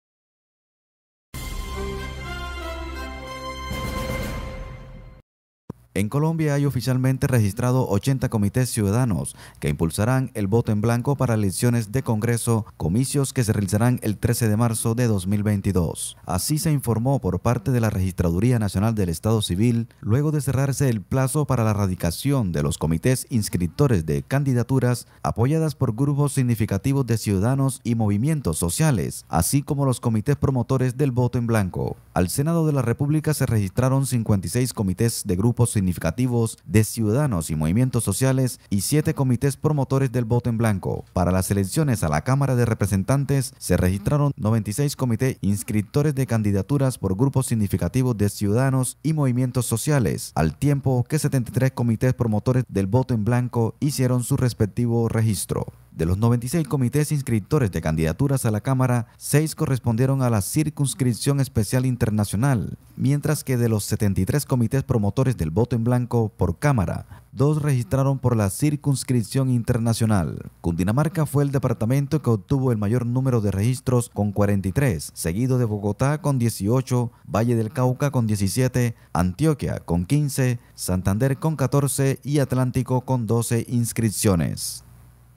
Se inscribieron 80 promotores del voto en blanco para elecciones de Congreso
En Colombia hay oficialmente registrados 80 comités ciudadanos que impulsarán el voto en blanco para las elecciones de Congreso en marzo, comicios que se realizarán el 13 de marzo de 2022.